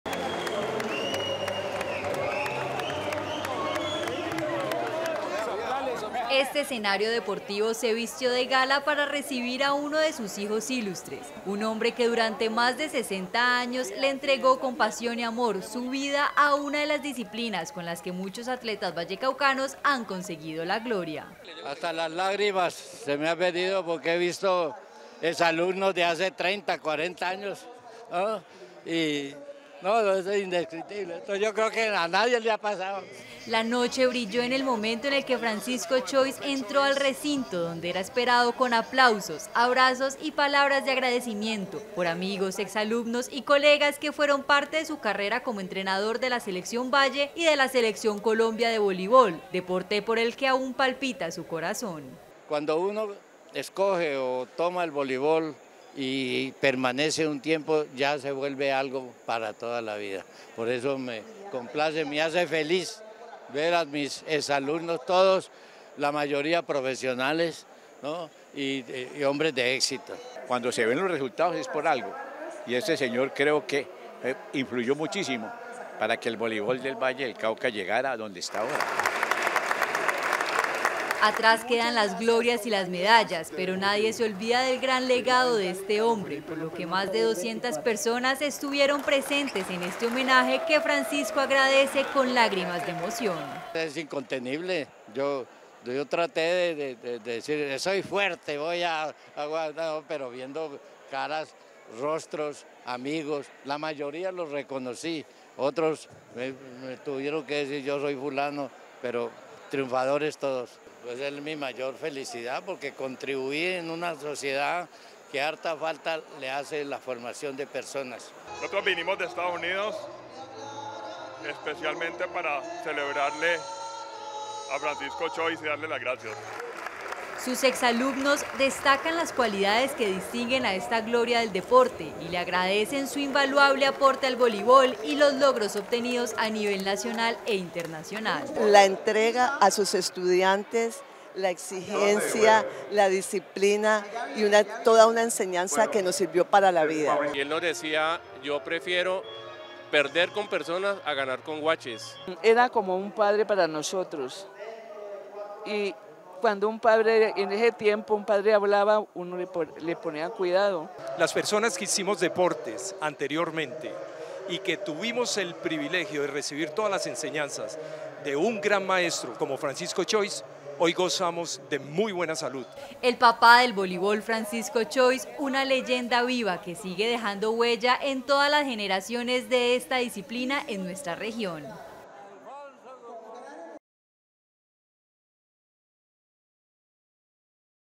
DEPORTES NOTI5: Homenaje al reconocido ex-entrenador de voleibol del Valle, Francisco Chois.
Informa: María Carolina García